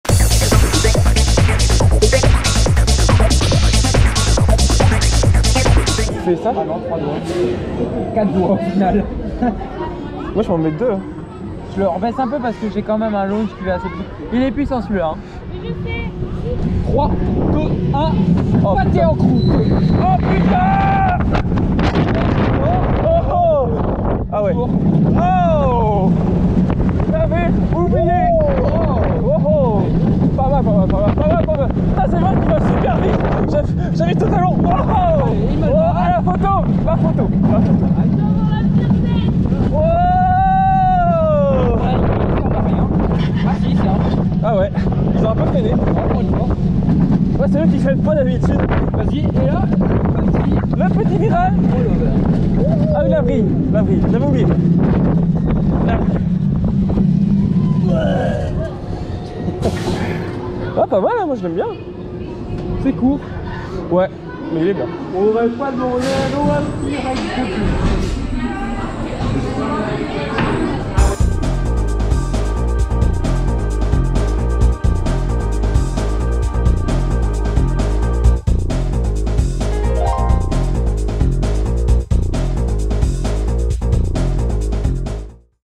ANUBIS (OnRide) | Plopsaland De Panne (2022)
Bonsoir à tous ! OnRide sur l'Anubis, à Plopsaland De Panne (2022)

Bonne vidéo !